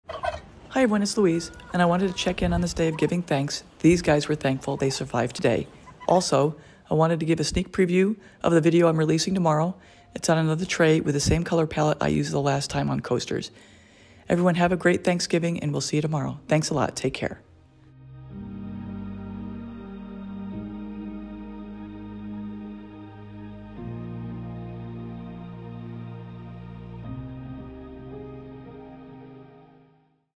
Happy Thanksgiving with sneak preview of a gorgeous tray creation video releasing tomorrow!
Hi Everyone,
I just want to say to everyone how thankful I am to have you joining me in this acrylic paint pouring journey. You have given me inspiration to dive deeper into this artform and continue to challenge myself with various creations and techniques. I never would have thought a year ago, I'd have a YouTube channel, 2 years ago I would have spelled YouTube, Utube, yeah, that's where I was, and paint pouring, never heard of it. So, yes we all change and grow, that's life!
Enjoy your Thanksgiving, think about all that you have or had, it's what made you what you are today,  and always enjoy the moment as much as possible!
We'll see you tomorrow. Leave comments when you can, I love to read what you're thinking.
Take care,
Louise